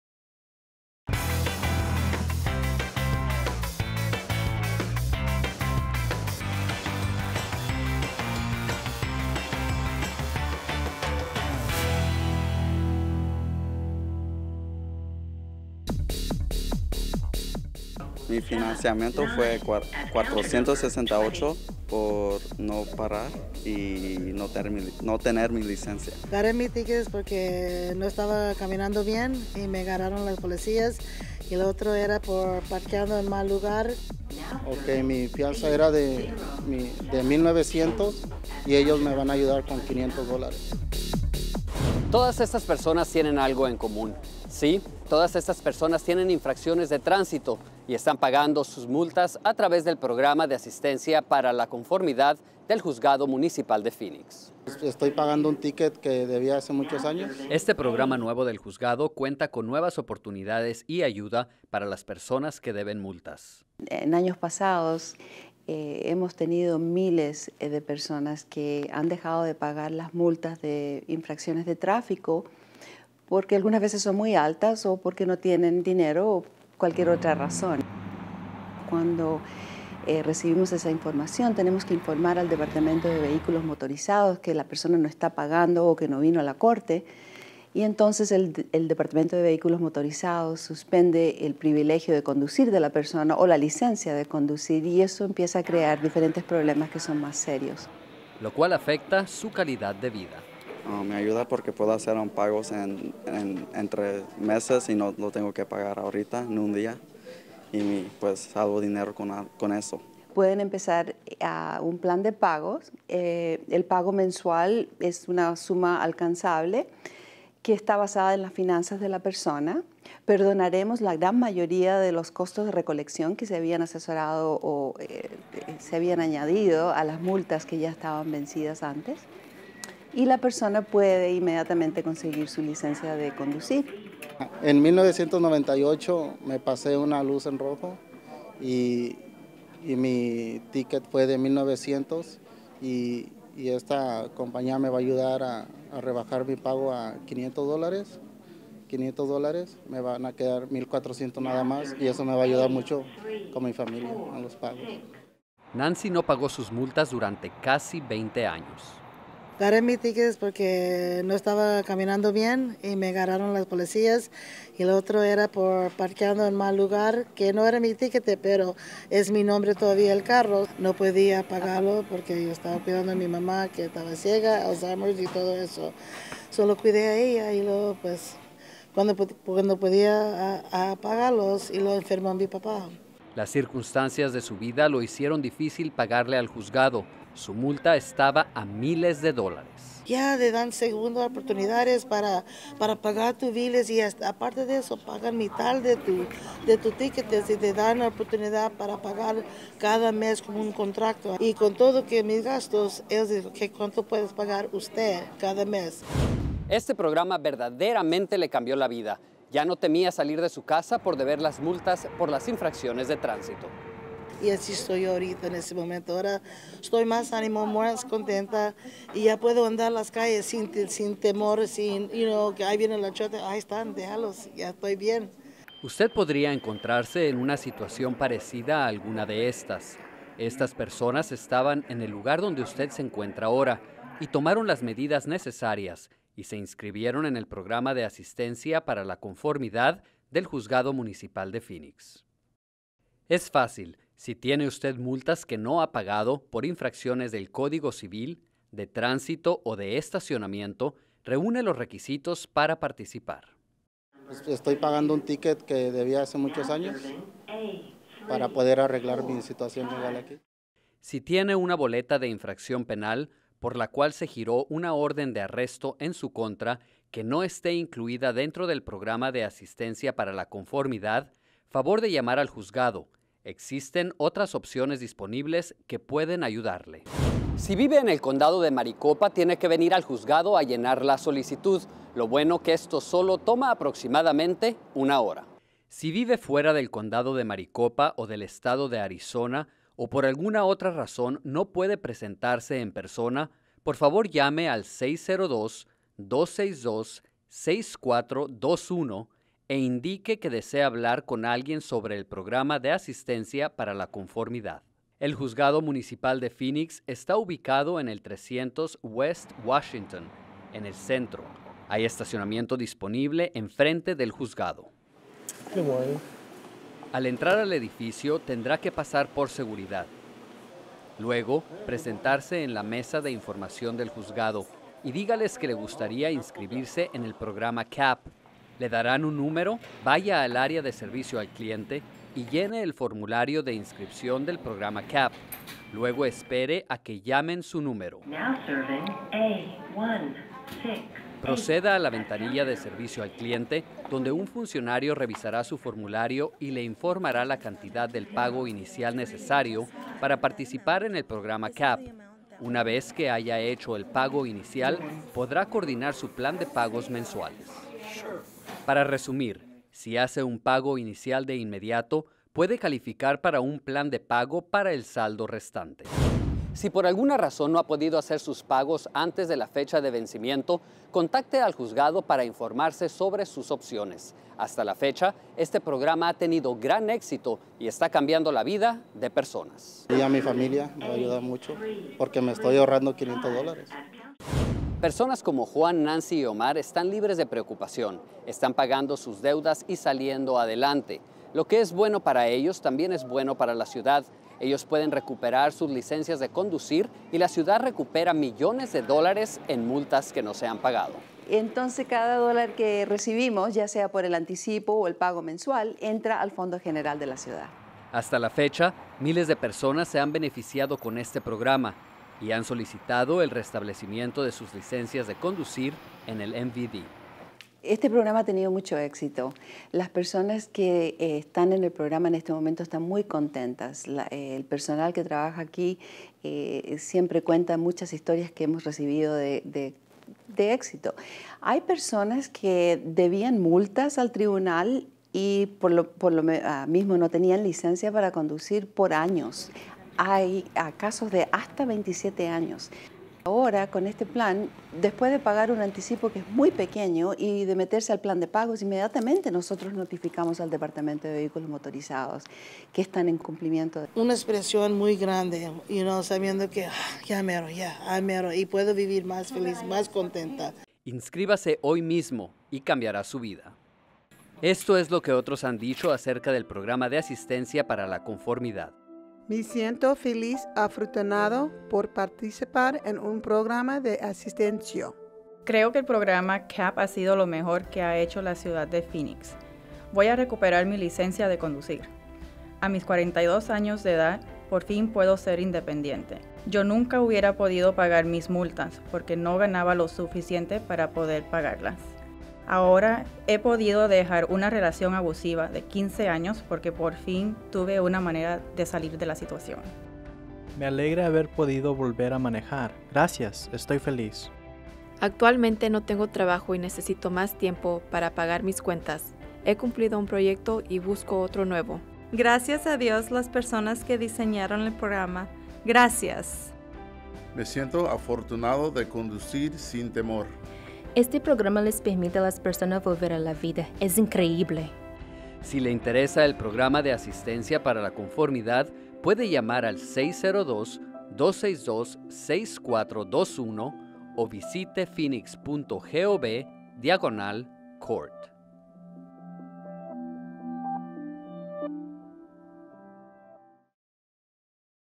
Tribunal Municipal de Phoenix / CAP
El Programa de Asistencia para la Conformidad (CAP) es un nuevo programa propuesto por el Tribunal Municipal de Phoenix. El CAP está proyectado
para ayudarle a solucionar multas de tránsito vencidas que se adeudan al Tribunal que pueden estar impidiendo el restablecimiento de su licencia de
conductor de Arizona, para hacer un plan de pagos por todo cargo de tránsito y de estacionamiento en lo civil, sin tener que comparecer ante un juez.
Si paga el anticipo y está de acuerdo en hacer pagos mensuales accesibles, el Tribunal le notificará al Departamento de Vehículos Motorizados de Arizona
(MVD - Por sus siglas en Inglés) que usted está en conformidad con el Tribunal con las multas y cuotas de tránsito en lo civil. Entonces puede comunicarse
con el MVD para averiguar qué debe hacer para restablecer sus privilegios de conducir.